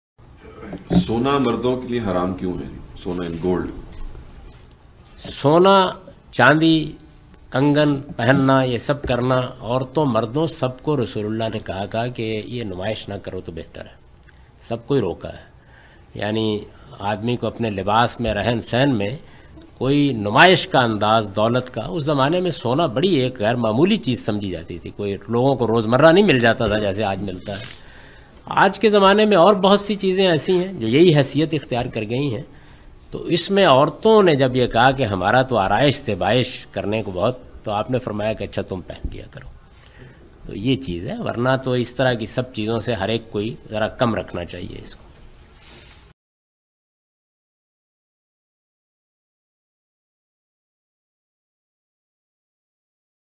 Why can't men wear gold but women can? | Javed Ahmad Ghamidi
Questions like, "why can't men wear gold?" or "why can women wear gold while men can't?" are itching ones in the minds of today's youth; and so Javed Ahmad Ghamidi clarifies after a question in his question and answer session.